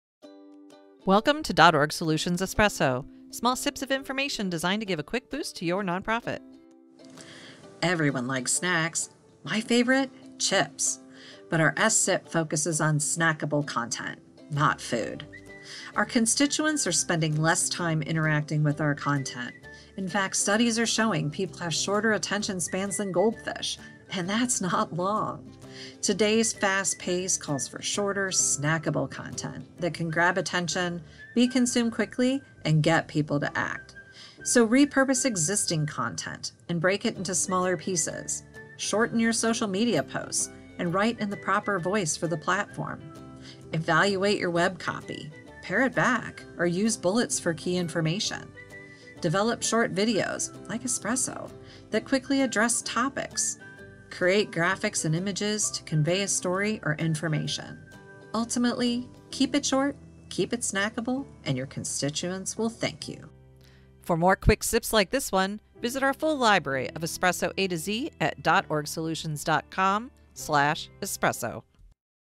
S is for Snackable Content
In today’s fast-paced world, our attention spans are short, and we’re spending more time scanning than reading. That’s why creating short and “snackable” content is the best way to hold your audience’s attention and inspire them to act!